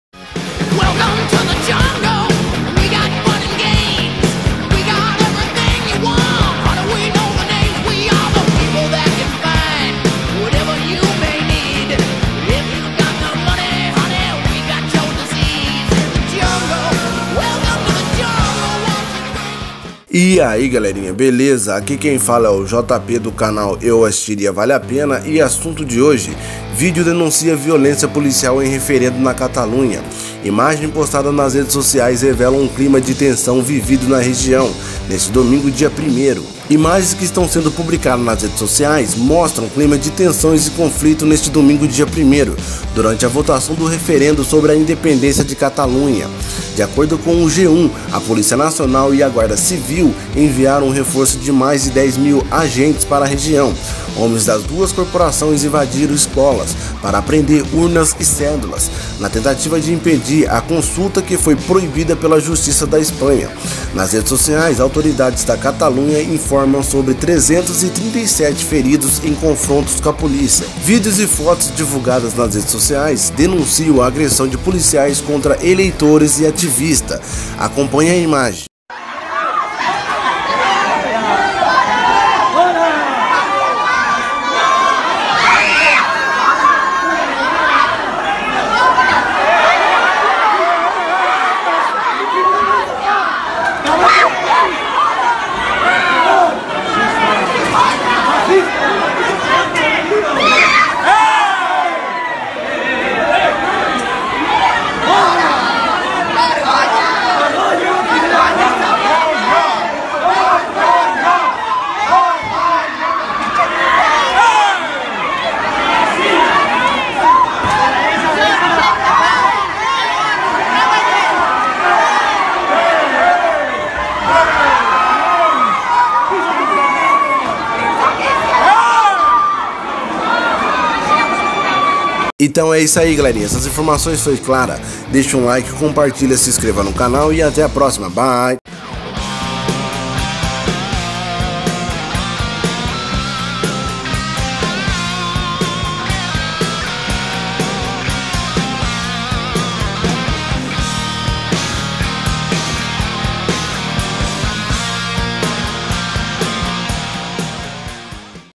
Denuncia de violência policial em referendo na Catalunha. ( Noticias e politica )
Mais de 340 pessoas foram atendidas por serviços médicos em decorrência da ação policial para reprimir o referendo pela independência da região autônoma da Catalunha, convocado por separatistas neste domingo 1/09/2017. Entre as vítimas, 337 eram civis e 11 eram agentes de segurança.